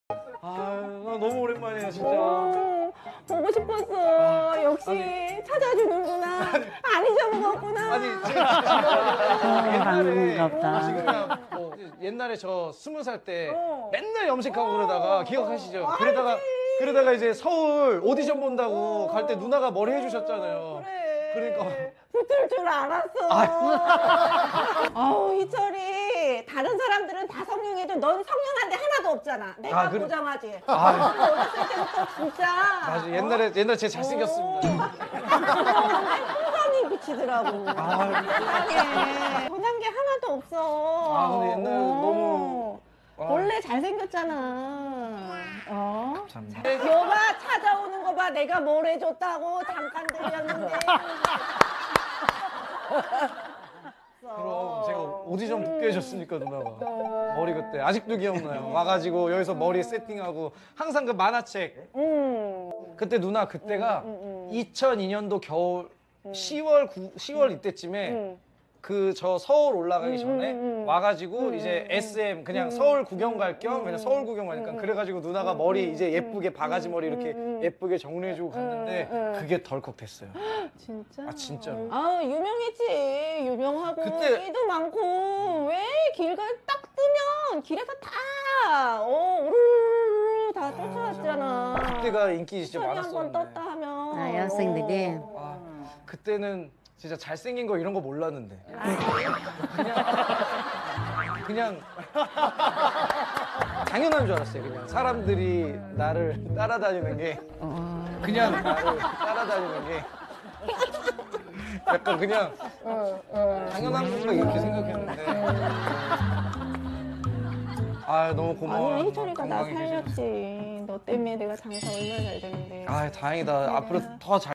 Kim Heechul Never Forgets | He Revisited the Salon Owner Who Helped Him Pass the SM Audition
Despite being incredibly busy and famous, in 2019, Kim Heechul paid a visit and thanked the salon owner who helped him a lot when he was starting out. He was really humble about it and acknowledged how much the stylist did to help him succeed. By talking about this on his show, he also gave the stylist some well-deserved recognition and publicity. It really shows that Heechul cares about and respects the people who helped him get to where he is today. Kim Heechul is truly a good person! Good guy Kim Heechul.
Good guy Kim Heechul.

우주대스타 KimHeeChul Heechul 김희철 金希澈 🍒🐱🐶💙